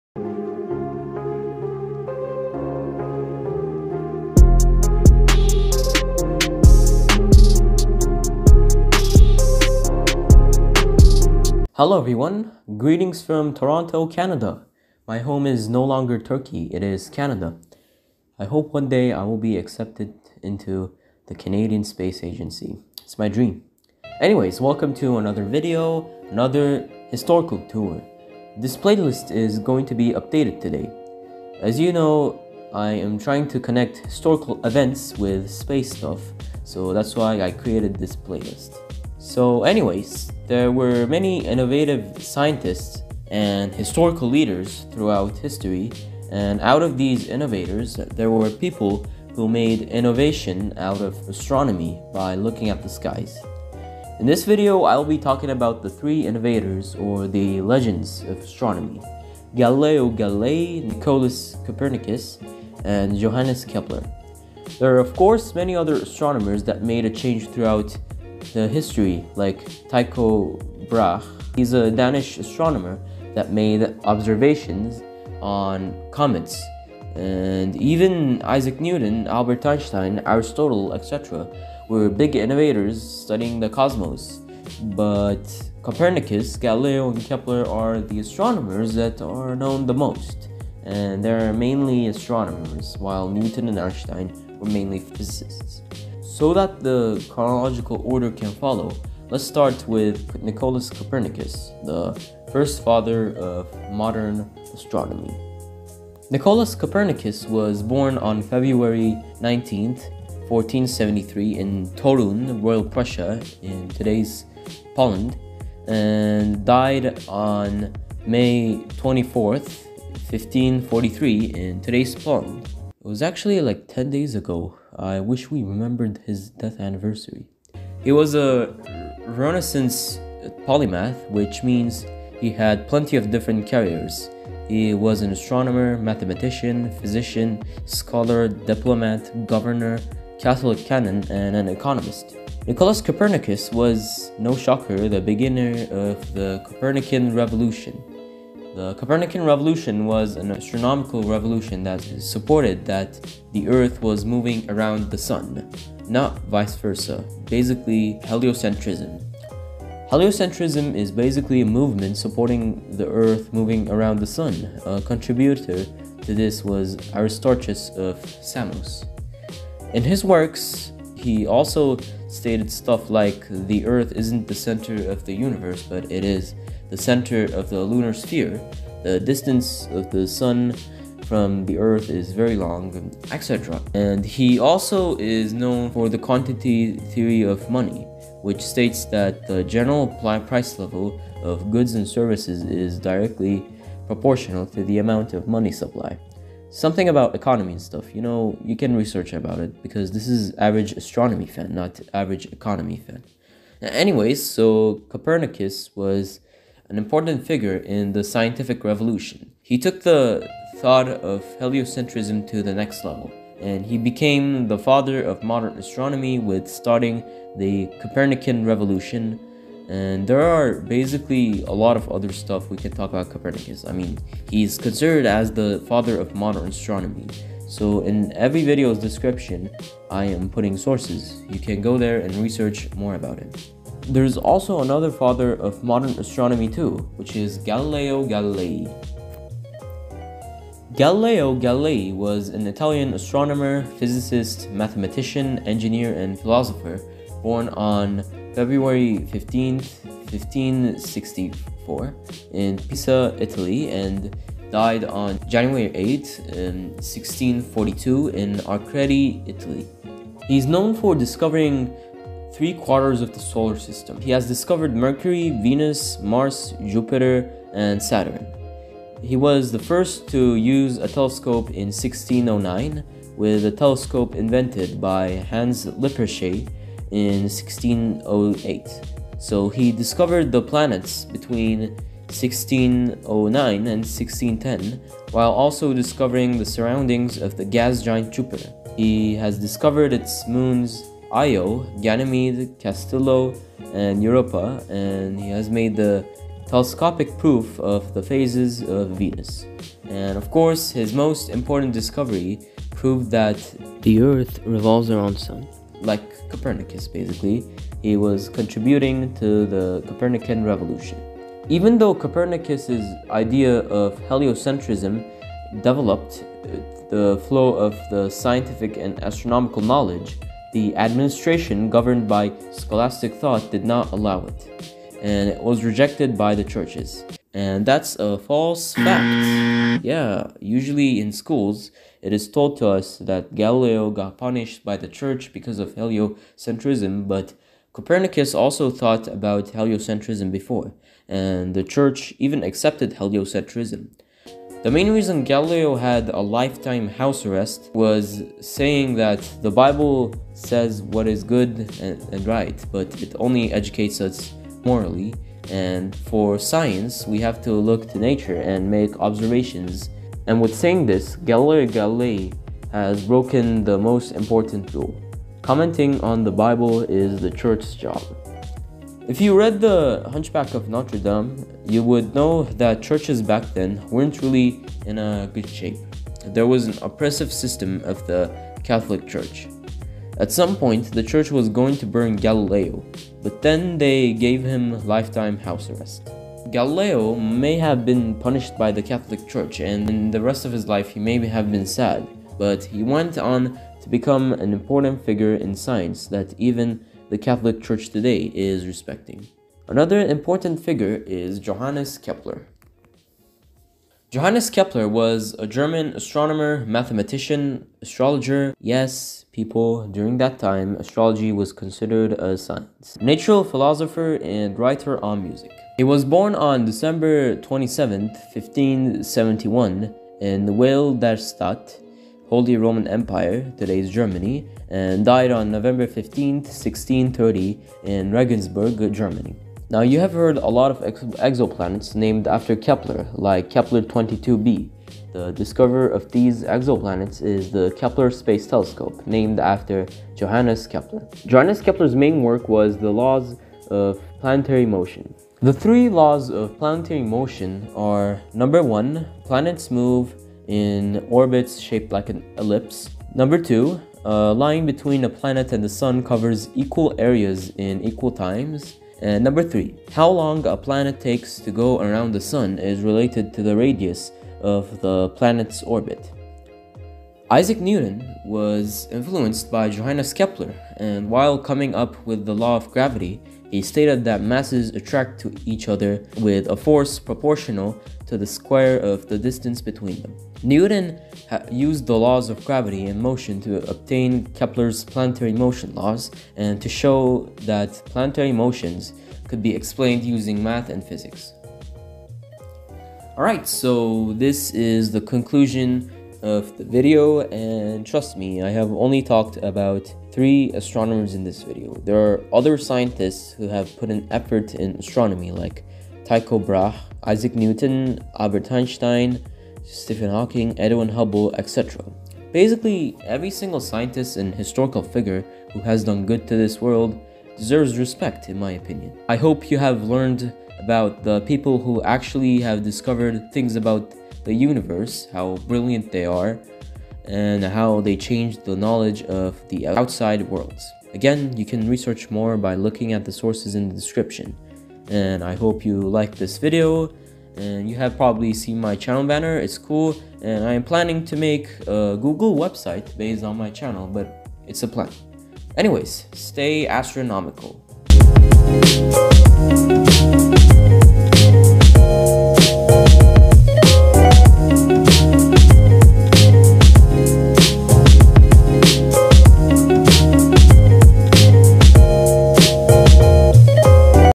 The Three Legends of Astronomy: Galileo Galilei, Nicolaus Copernicus and Johannes Kepler
Hello everyone, in this video, I have talked about the scientists of the Scientific Revolution, Galileo, Copernicus and Kepler, examples of astronomers that contributed a lot on astronomy. Hope you liked it, stay astronomical!

00:00 Introduction
01:45 Nicolaus Copernicus
03:53 Galileo Galilei
06:39 Johannes Kepler
08:12 Conclusion